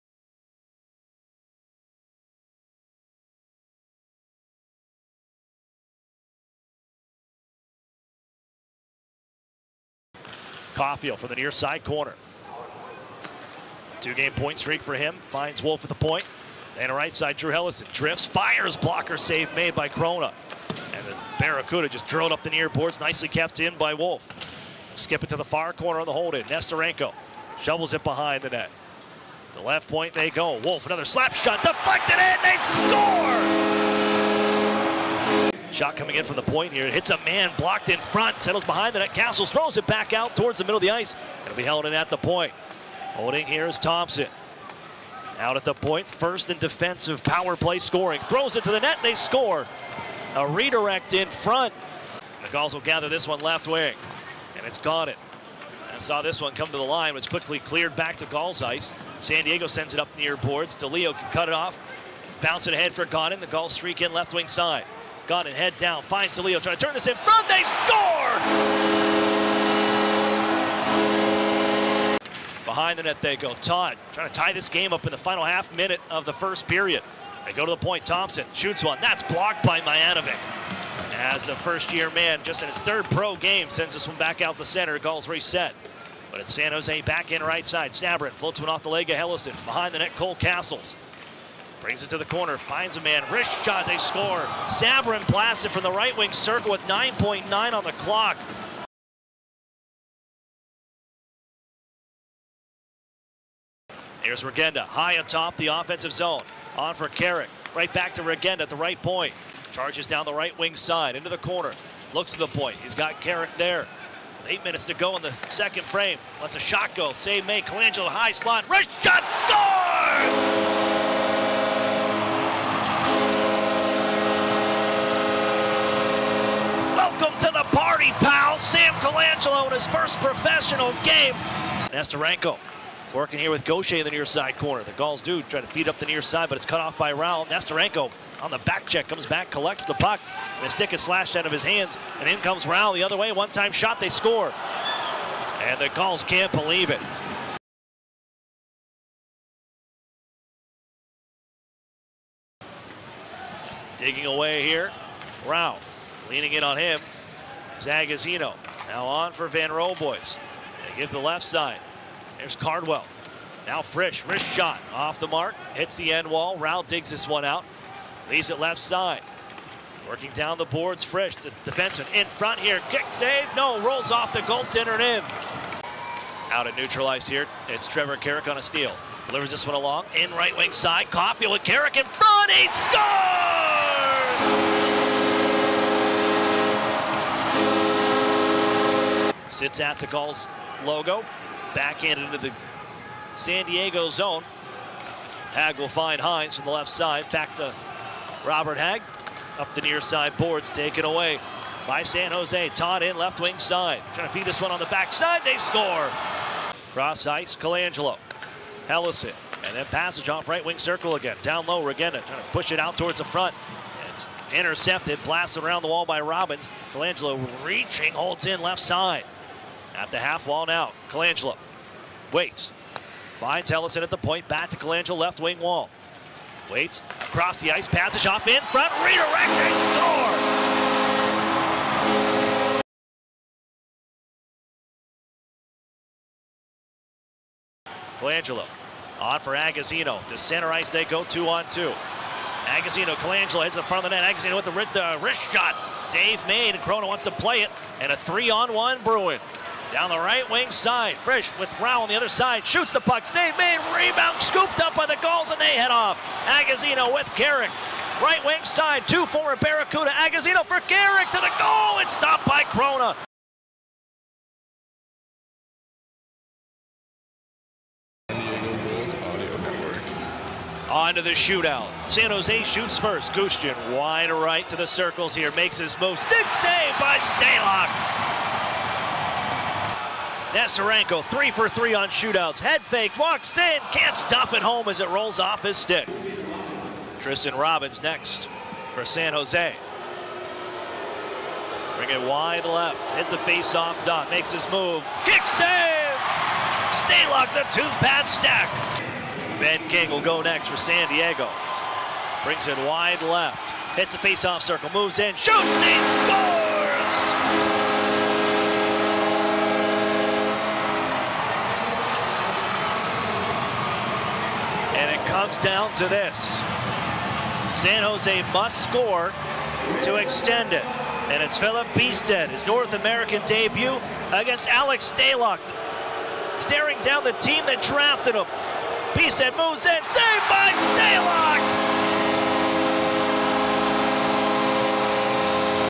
Recap: San Diego Gulls vs. San Jose Barracuda, 4.3
The Gulls get the win in an exciting shootout with the San Jose Barracuda. Sam Colangelo makes his professional debut, tallying two points, one goal and one assist. Alex Stalock held it down in net, not letting a single San Jose Shootout attempt past him.